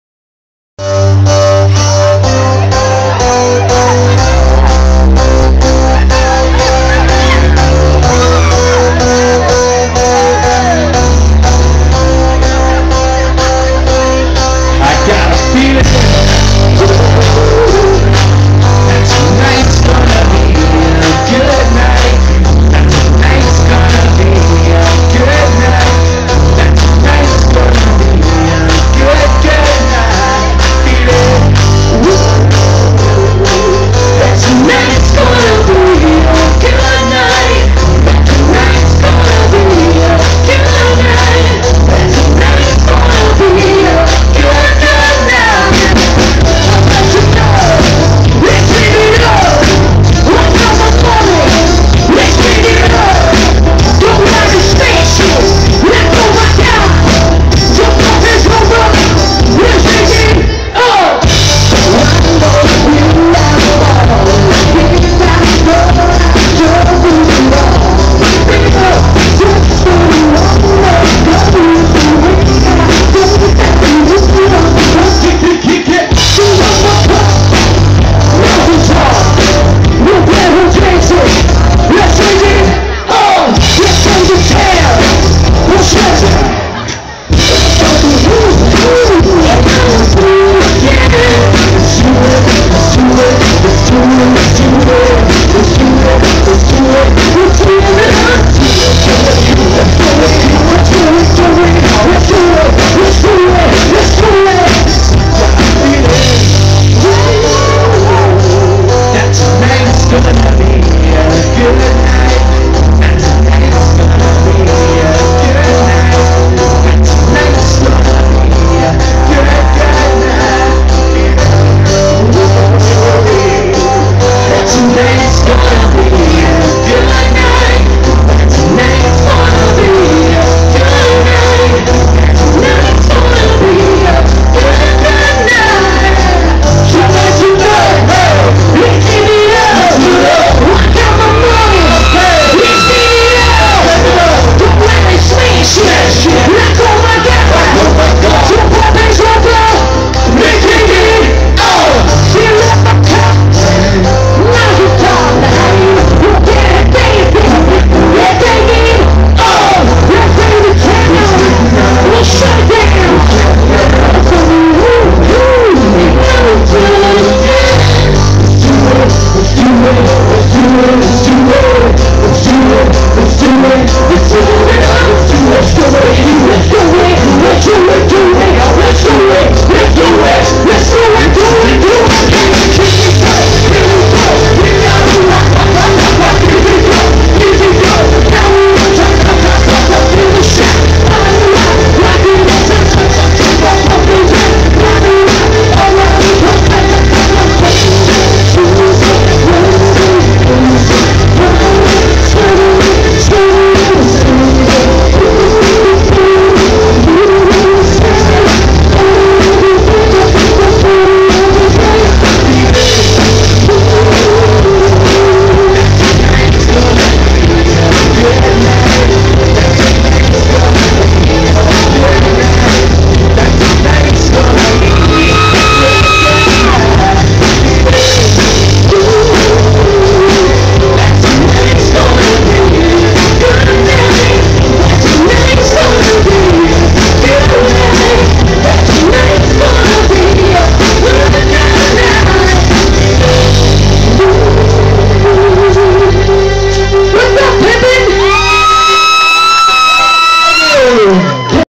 Switch
Switch playing at The Mount Pleasant, PA Glass Festavial.(Sorry about the sound I stood too close to the speakers.)